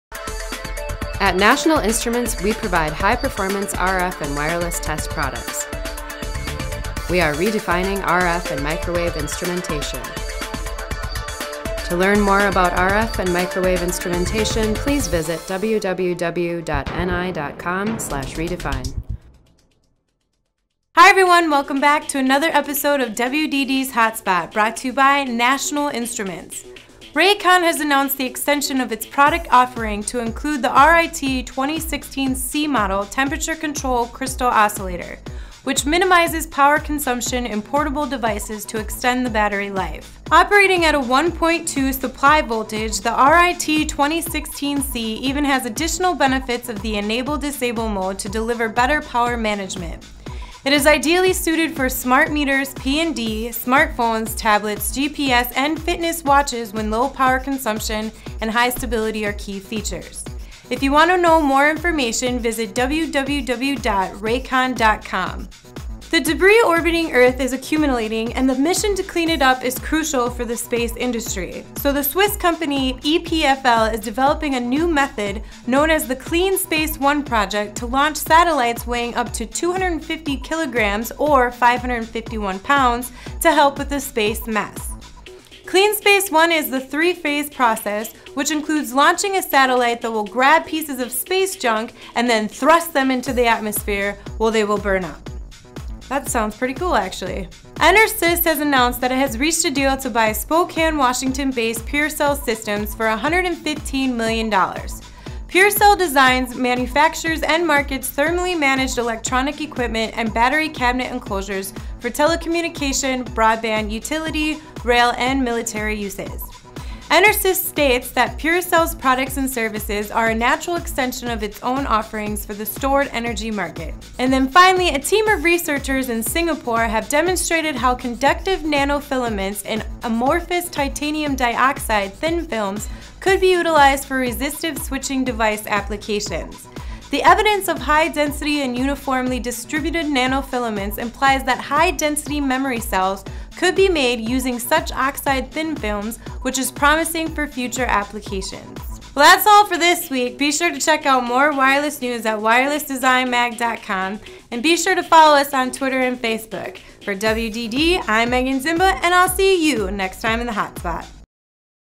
HotSpot Episode 31: Cleaning Up Space Mess
-- Rakon has announced the extension of its product offering to include the RIT2016C model Temperature-Controlled Crystal Oscillator, which minimizes power consumption in portable devices to extend the battery life. Operating at a 1.2 V supply voltage, the RIT2016C even has additional benefit of the enable-disable mode to deliver better power management. It is ideally suited for Smart Meters, PND, Smartphones, Tablets, GPS and Fitness Watches when low power consumption and high stability are key features.

-- The debris orbiting Earth is accumulating, and the mission to clean it up is crucial for the space industry, so the Swiss company EPFL is developing a new method, known as the CleanSpace One project, to launch satellites weighing up to 250 kg, or 551 pounds, to help with the space mess. CleanSpace One is a three-phase process, which includes launching a satellite that will grab pieces of space junk, and then thrust them into the atmosphere, where they will burn up.

-- EnerSys has announced that it has reached a deal to buy Spokane, Washington-based Purcell Systems for $115 million. Purcell designs, manufactures, and markets thermally-managed electronic equipment and battery cabinet enclosures for telecommunication, broadband, utility, rail, and military uses. EnerSys states that Purcell's products and services are a natural extension of its own offerings for the stored energy market.

-- A team of researchers in Singapore have demonstrated how conductive nano-filaments in amorphous titanium dioxide (TiO2) thin films could be utilized for resistive switching device applications. The evidence of high density and uniformly distributed nano-filaments implies that high-density memory cells could be made using such oxide thin films, which is promising for future applications.